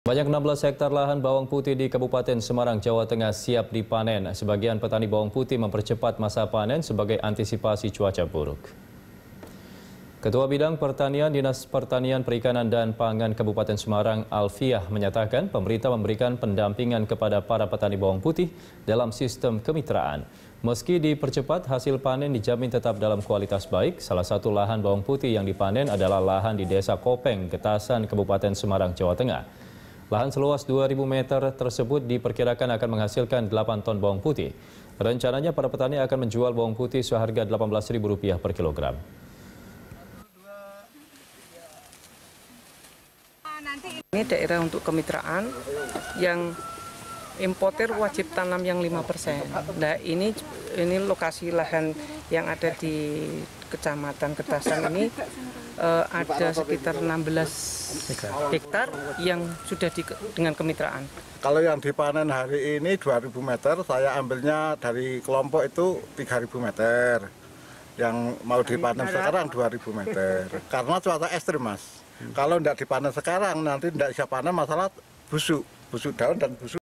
16 Hektare Lahan Bawang Putih di Semarang Siap Panen
Sebanyak 16 hektare lahan bawang putih di Kabupaten Semarang, Jawa Tengah, siap dipanen. Sebagian petani bawang putih mempercepat masa panen sebagai antisipasi cuaca buruk.

Facebook.com/BeritaSatu
Youtube.com/BeritaSatu